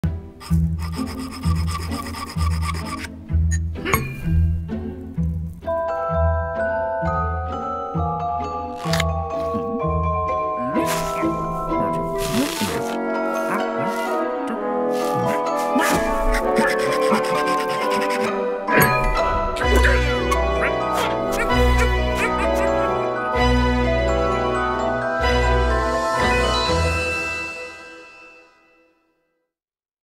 Happy Holidays Christmas Card - Deloitte Digital
Sound recorded and mixed at Risk Sound in Melbourne for Deloitte Digital.